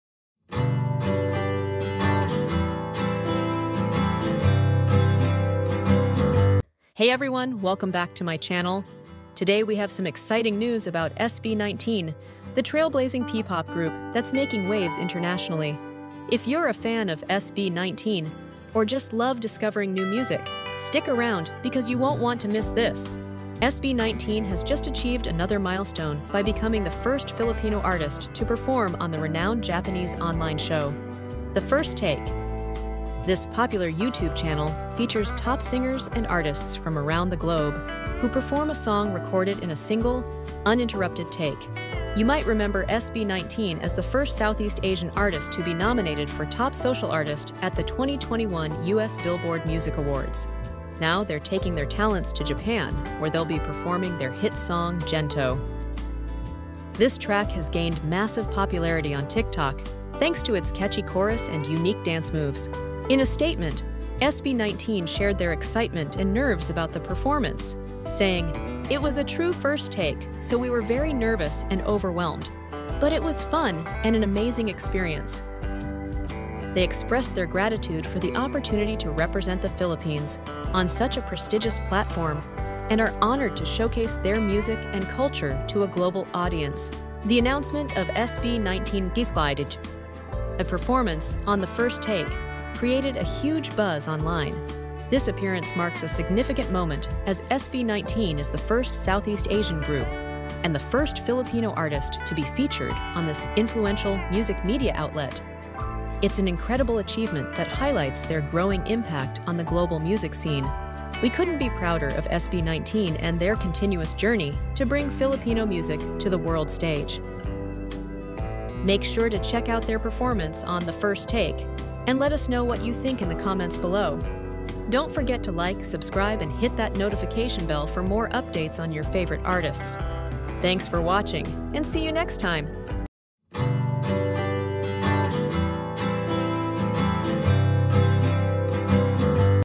SB19 Makes History on Japan’s 'The First Take' - First Filipino Artist
Hey everyone! 🎉 In today's video, we have some exciting news about SB19, the groundbreaking P-pop group that's making waves globally! 🌏

SB19 has just achieved a significant milestone by becoming the first Filipino artist to perform on the renowned Japanese online show, 'The First Take.' You won't believe what song they chose and how they felt about this incredible opportunity! 😲

Tune in to find out more about their historic performance and why this is such a big deal for Filipino music and culture. 🇵🇭